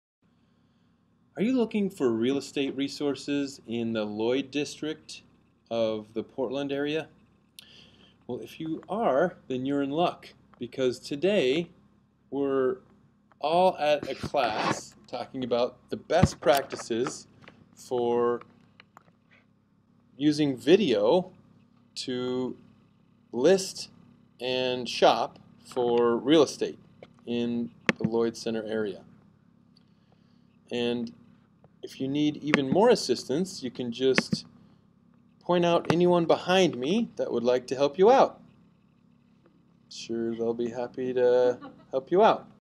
Real Estate Resources in Lloyd District - Portland, OR
This is a quick video on the best Real Estate Resources in Lloyd District - Portland, OR.

Lloyd Center Conference Room
825 NE Multnomah St
Portland, OR 97232